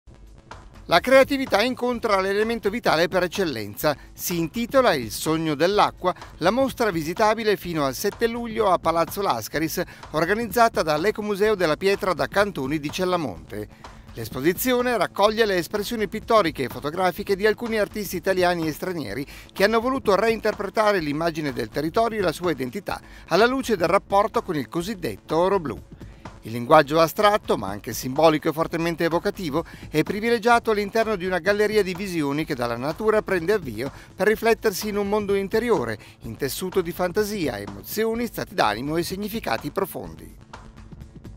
CrpTv n.17 del 01 giugno 2012 - "Il sogno dell'acqua"
La creatività incontra l'elemento vitale per eccellenza. Si intitola "Il sogno dell'acqua", la mostra visitabile fino al 7 luglio a Palazzo Lascaris, organizzata dall'Ecomuseo della pietra da cantoni di Cella Monte. L'esposizione raccoglie le espressioni pittoriche e fotografiche di alcuni artisti italiani e stranieri che hanno voluto reinterpretare l'immagine del territorio e la sua identità alla luce del rapporto con il cosiddetto "oro blu". Il linguaggio astratto, ma anche simbolico e fortemente evocativo, è privilegiato all'interno di una galleria di visioni che dalla natura prende avvio per riflettersi in un mondo interiore, intessuto di fantasia, emozioni, stati d'animo e significati profondi.